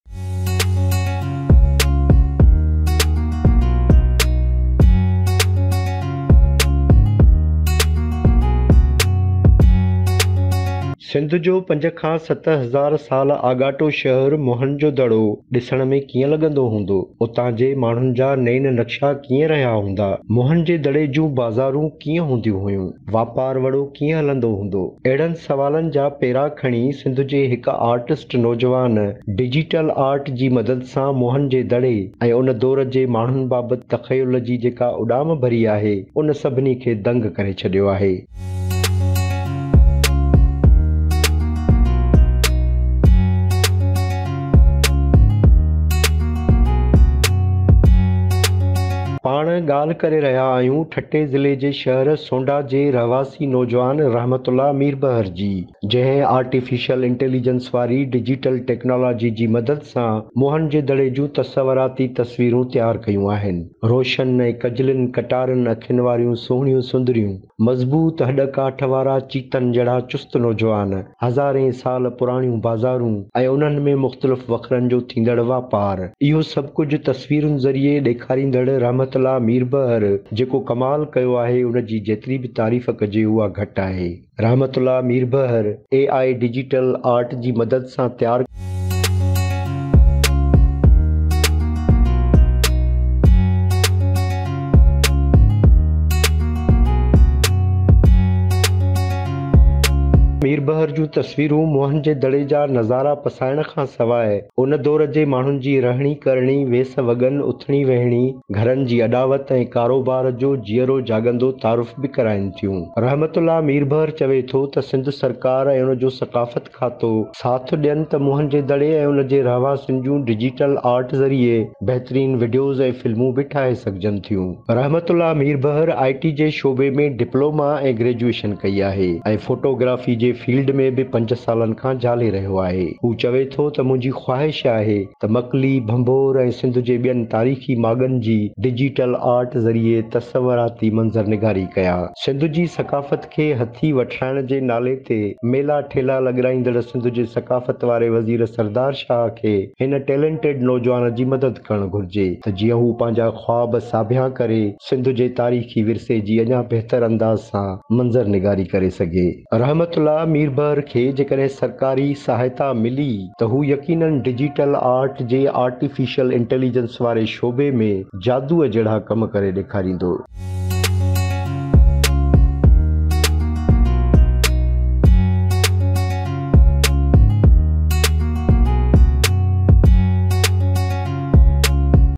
Sindh Jay Nojawan Artist Mohen Jo Daro Khay Beehar Jearao Karay Chadyo
Thatto jay young artist Rehmatullah Mirbahar artificial intelligence digital art zareay Mohenjo Daro khay beehar jearo karay chadyo. Darawarr News TV special rerpor.

نوجوان آرٽسٽ موهن جو دڙو کي جيئرو ڪري ڇڏيو